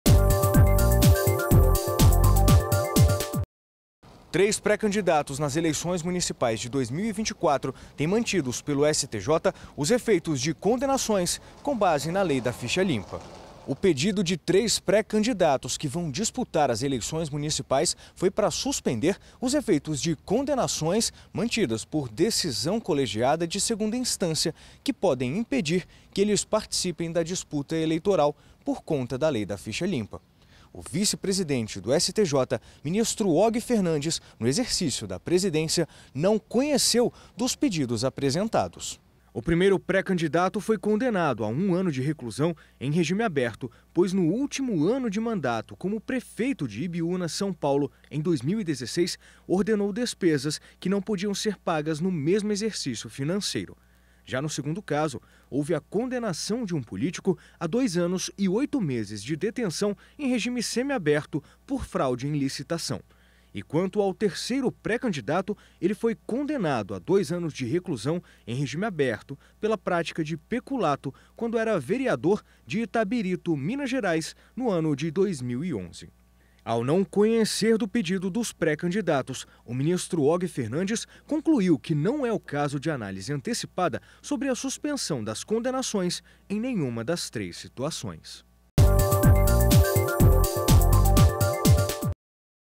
STJ mantém efeitos de condenações de três pré-candidatos às eleições municipais de 2024
​O vice-presidente do Superior Tribunal de Justiça (STJ), ministro Og Fernandes, no exercício da presidência, não conheceu do pedido de três pré-candidatos às eleições municipais de 2024 para suspender os efeitos das respectivas condenações que podem impedir que eles participem da disputa eleitoral em razão da Lei da Ficha Limpa.
TutCautAnt 571
TutCautAnt 572
HC 912534